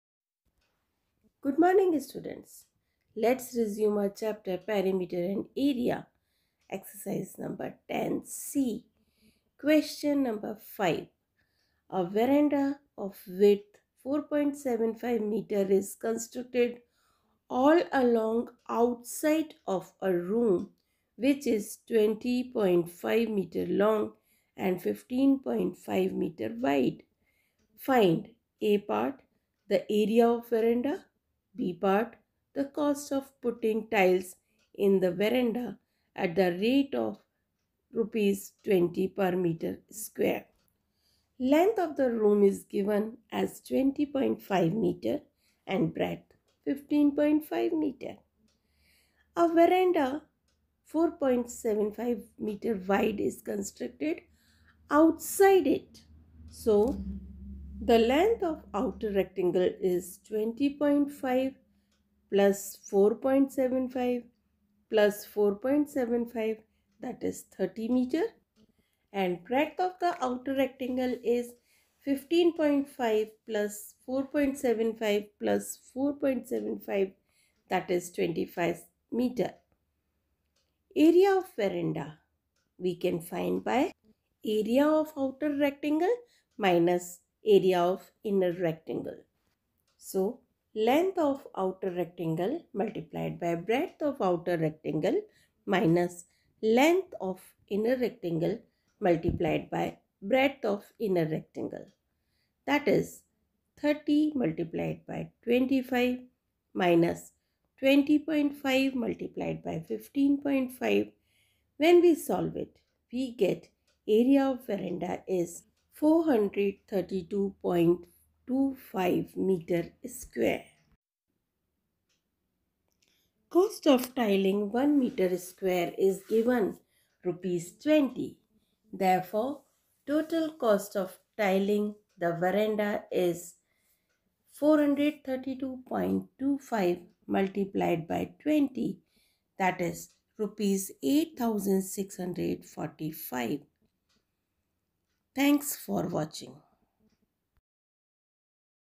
Class 7 : Perimeter and Area : Area of Verandah All Along Outside A Room
This video contains Q. no. 5 of exercise 10 C from Together With Mathematics Buzz of RACHNA SAGAR publication.